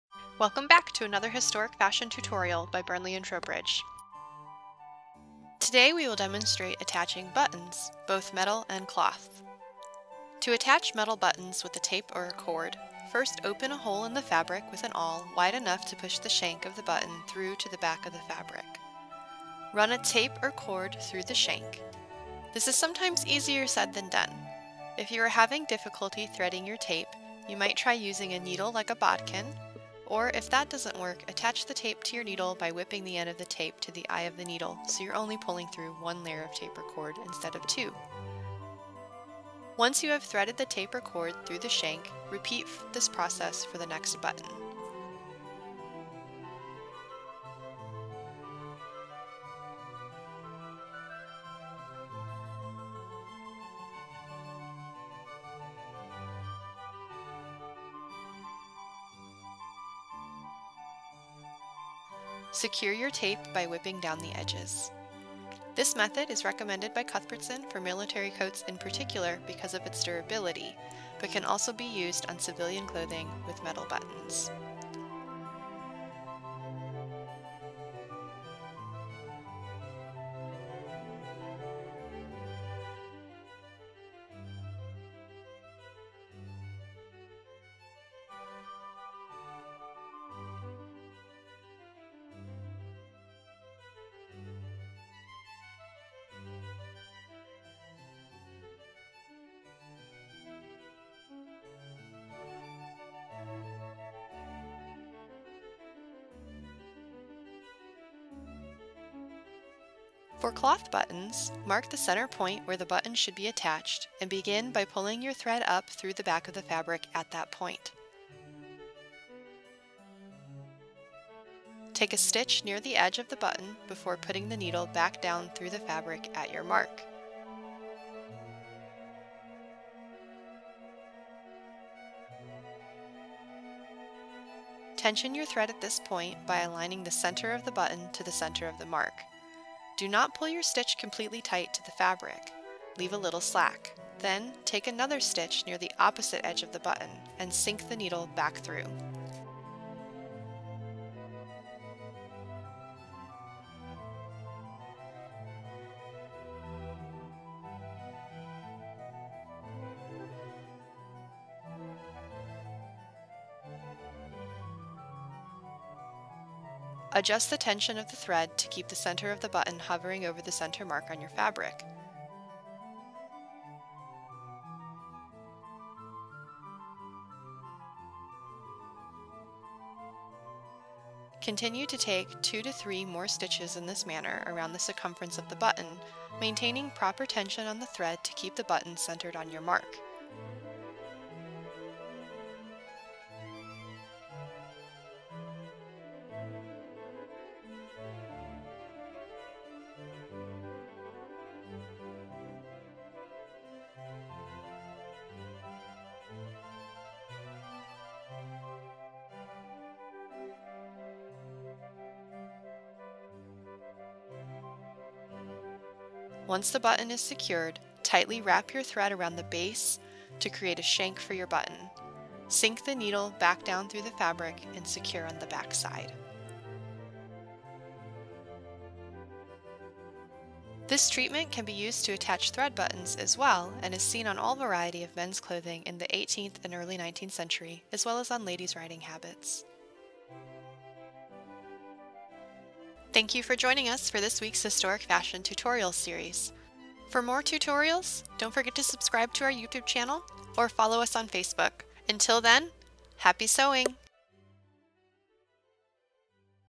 Historic Fashion Tutorial Series: Attaching Buttons
In this tutorial we will demonstrate how to attach metal buttons with a tape or lace and cloth buttons by creating a self shank.